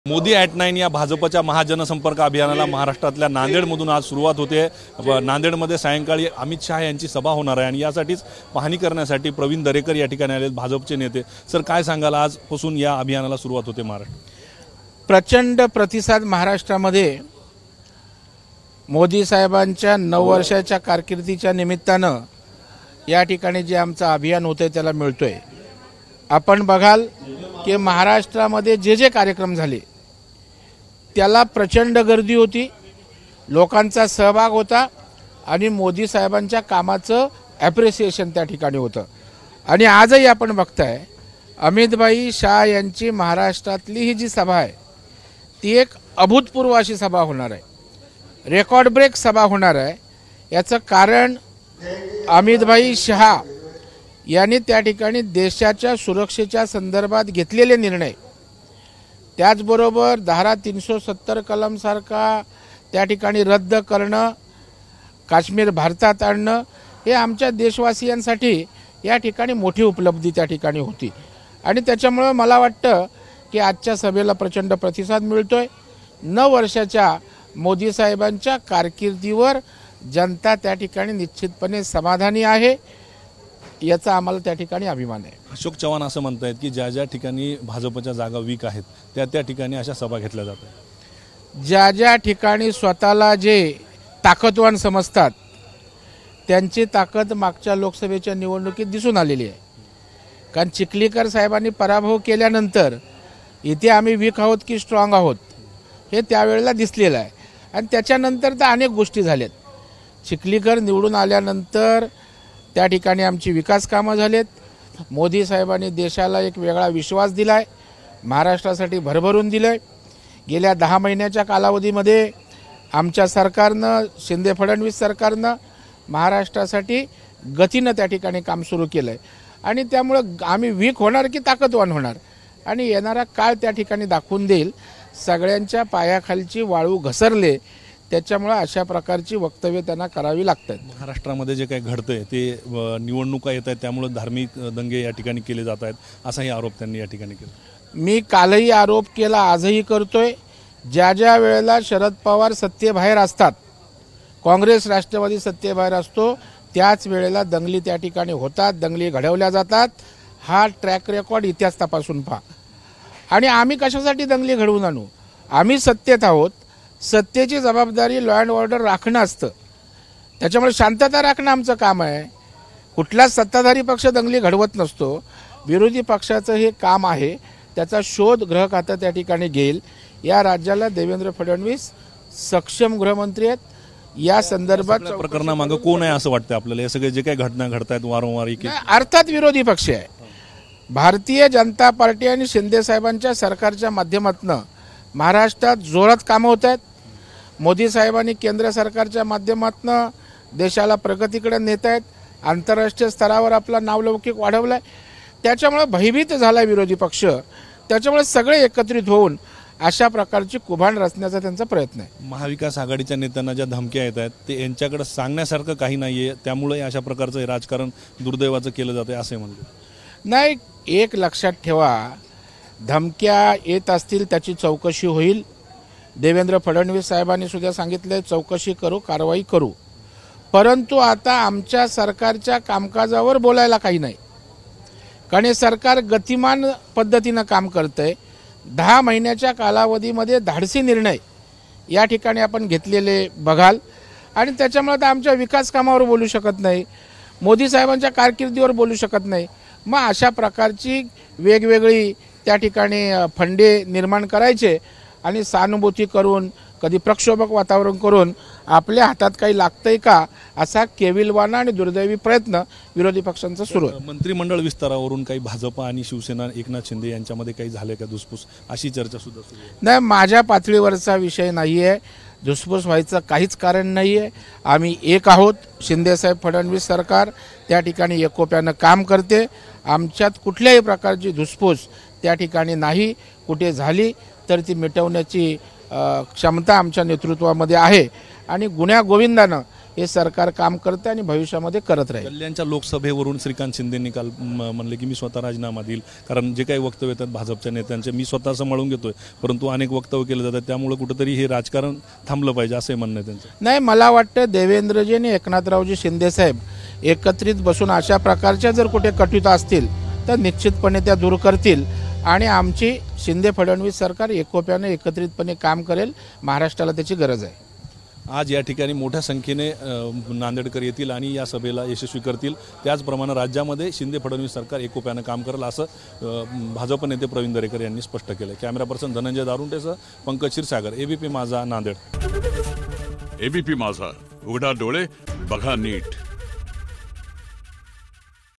Nanded Pravin Darekar : आम्ही सत्तेत आहोत, शांतता राखणं आमचं काम आहे : प्रवीण दरेकर
Video Credit CMeknathshinde Maharashtra Political Siddhi Sontakke / Producer Saurabh Bhole / Editor